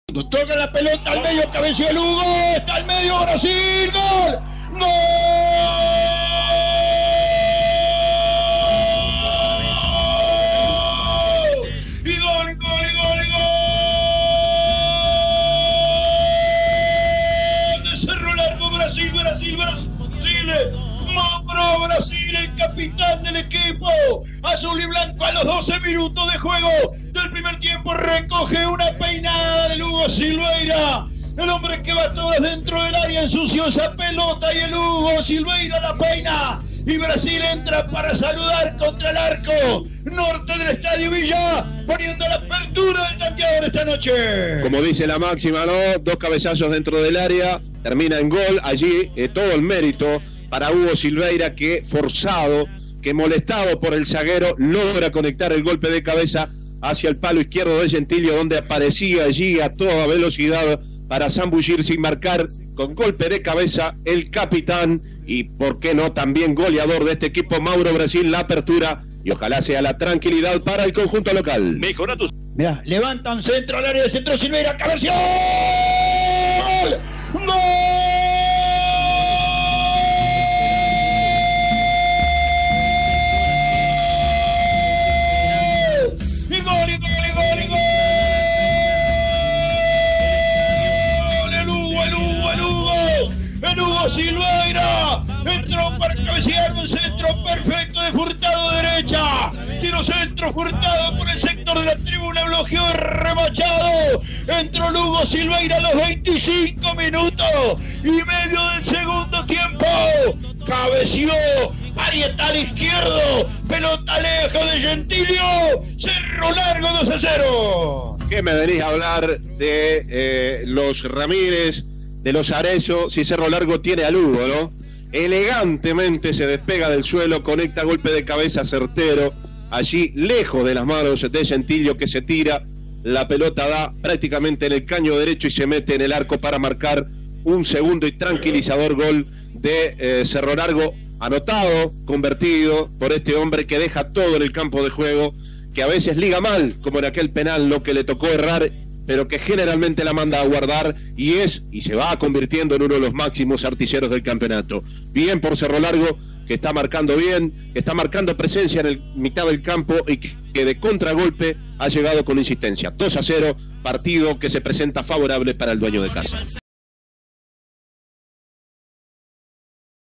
Cerro Largo FC 2 Racing 1 - Apertura 2023 (por Roberto Silva)
Los goles arachanes, por Roberto Silva y Gerardo Vidales para Radio Río Branco.